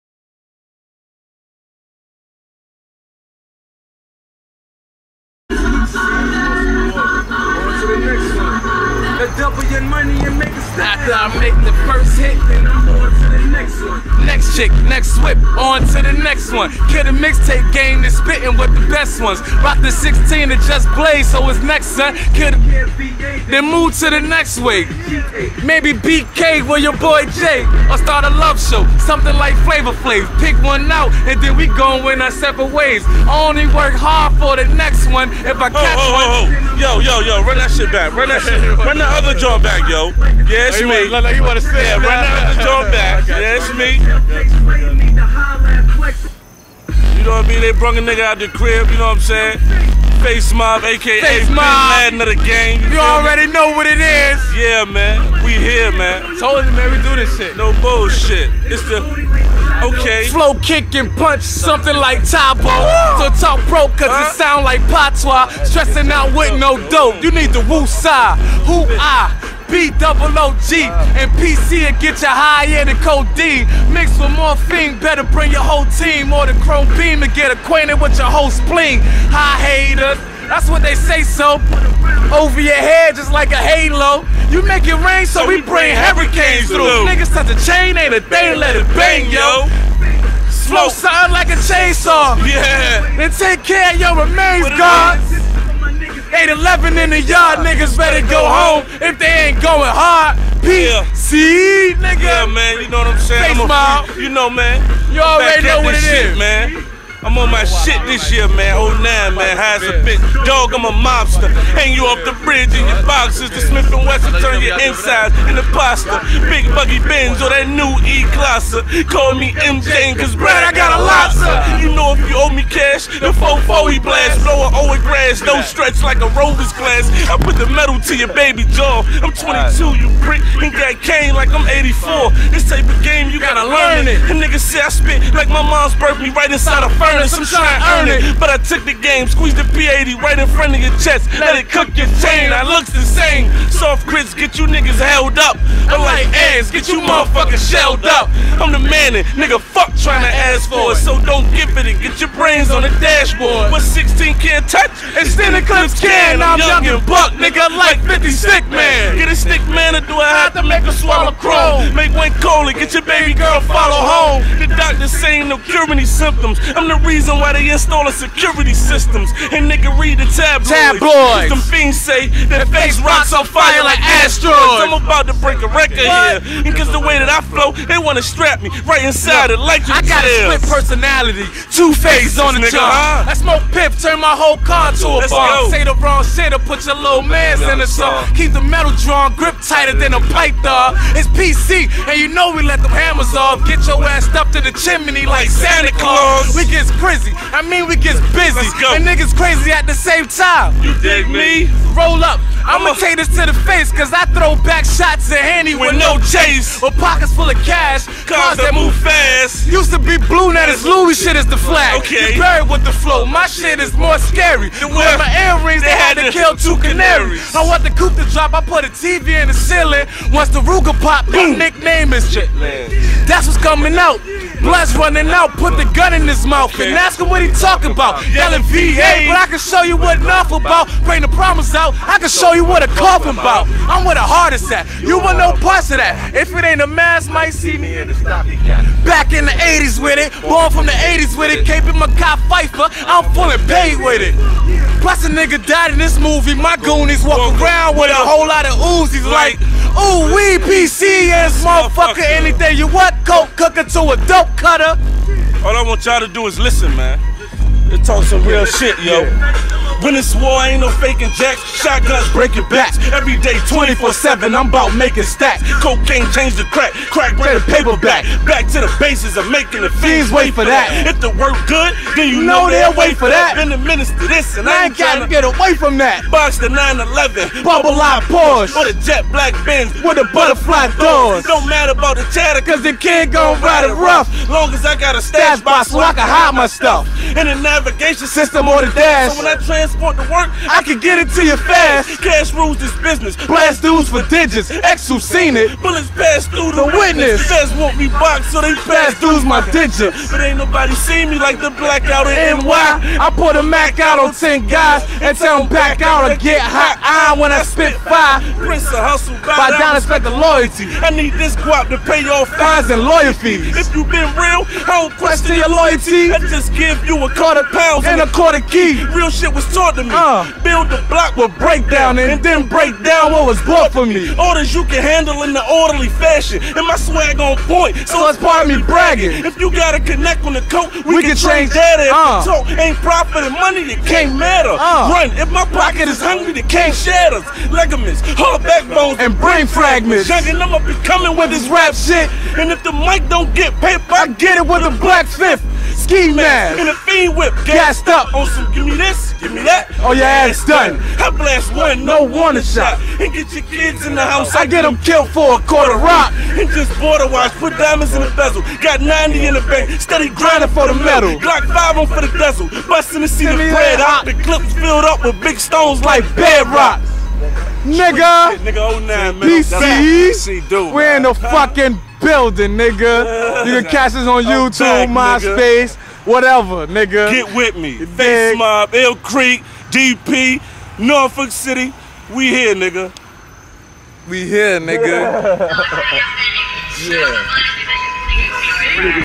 Outside E-Way crib PC Rappin Face Boogie
Gettin Ready to Go to a show at the Entourage in Norfolk . Boog Face rappin. Killin the game as usual. Come see the team every monday at entourage ya dig...